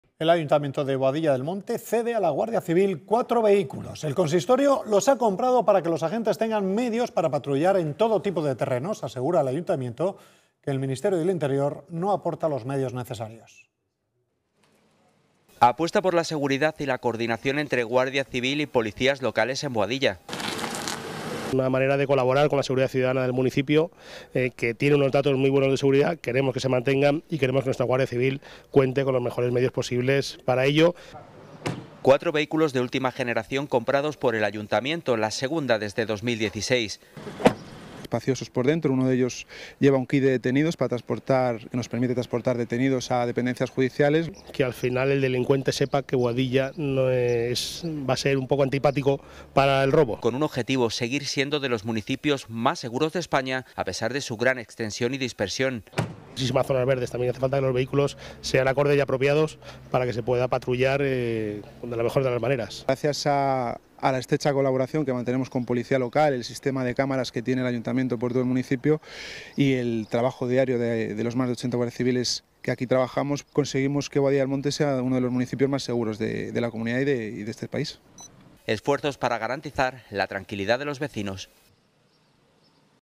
Cesión de coches a la Guardia Civil
Las cámaras se hacen eco de los coches que desde el ayuntamiento hemos puesto a disposición del puesto de la Guardia Civil de Boadilla.